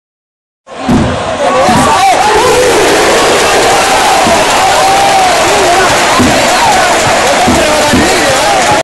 FABRICE OLINGA, Taconazo de Champions
El joven jugador estuvo a punto de marcar en Previa